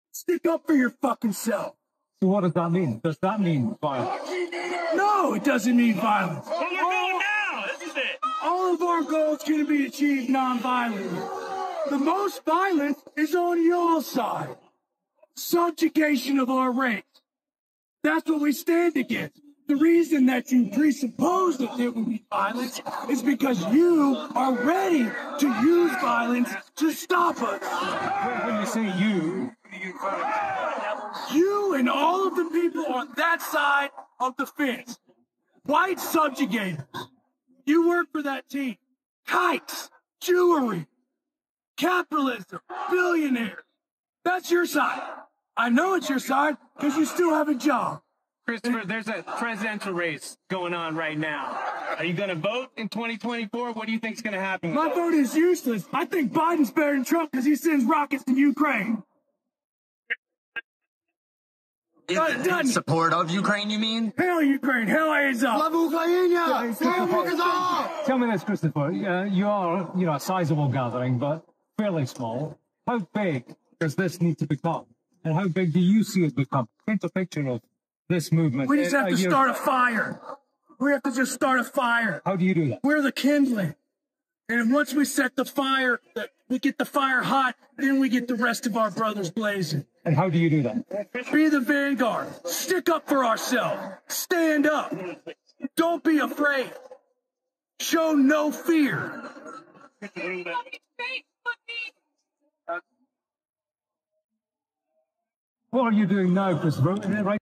Supports Biden In 2024 Over Trump Because Of Ukraine
He says he supports Biden in 2024 over Trump because of Ukraine.

What do you make of this?